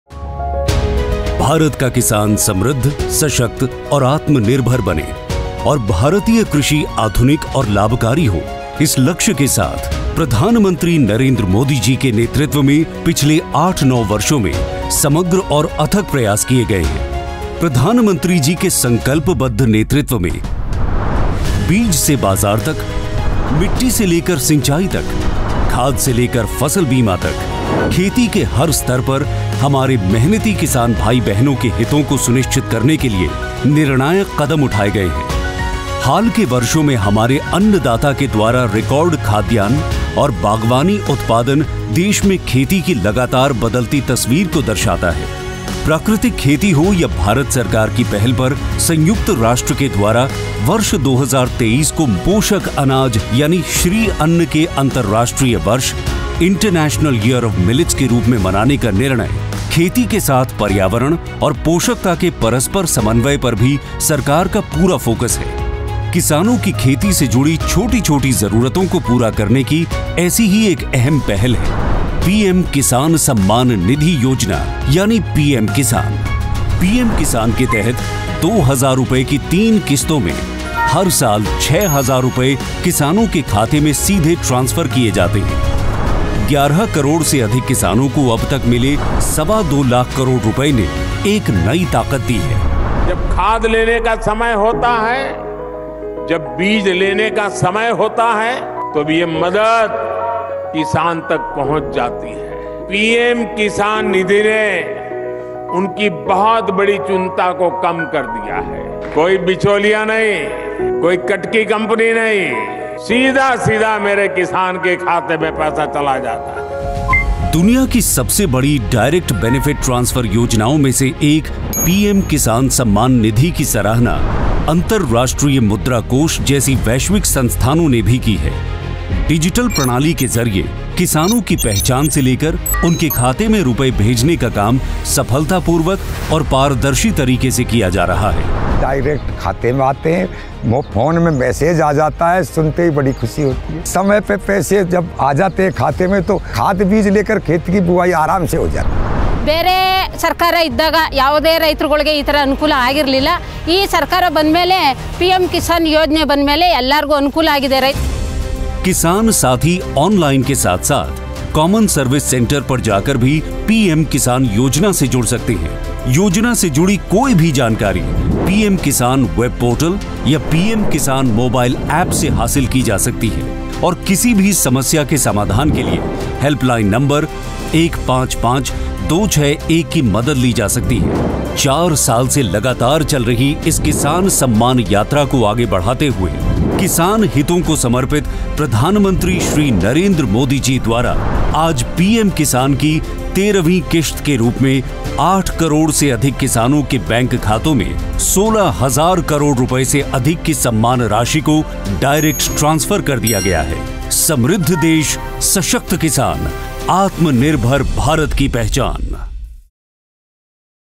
13th instalment amount of about Rs.16,000 crores under PM-KISAN released by PM Modi – Details here
In a step that will showcase yet another example of commitment of Prime Minister Narendra Modi towards welfare of farmers, the 13th instalment amount of about Rs. 16,000 crores under the Pradhan Mantri Kisan Samman Nidhi (PM-KISAN), has been released through direct benefits transfer to more than 8 crore beneficiaries. Under the scheme, eligible farmer families are provided a benefit of Rs. 6000 per year in three equal instalments of Rs. 2000 each.